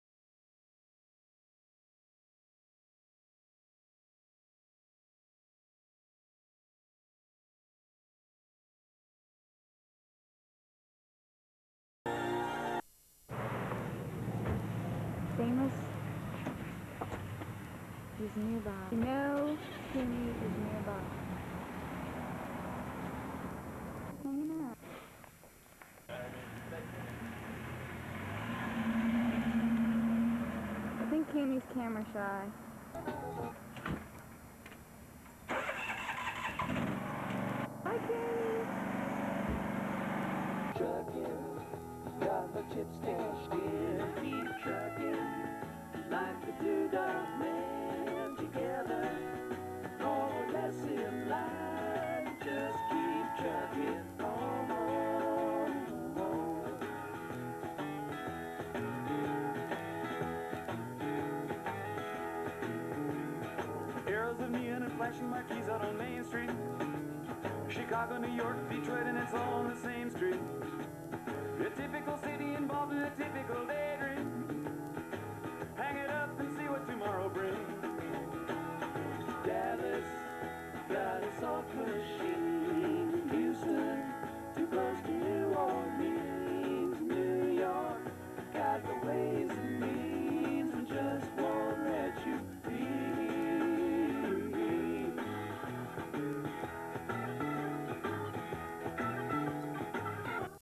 welcome to my wacky world XO
If you went to Western Carolina University during the 70-90's you have a story about Coony. Coony has since passed on, but when he was there he rode a tractor around campus and was allowed to go anywhere he wanted. It was rumored basically factual that Coony was an inbreed from Cullowhee, NC meaning his parents were bro and sis. RIP -COONY

He lived a great life. he first started out with a red tractor but our baseball team bought him a John Derre and he was known to cut a row of grass in a field and say look what I did. My college is where they filmed Deliverence and used the locals. Enjoy... flashback 1990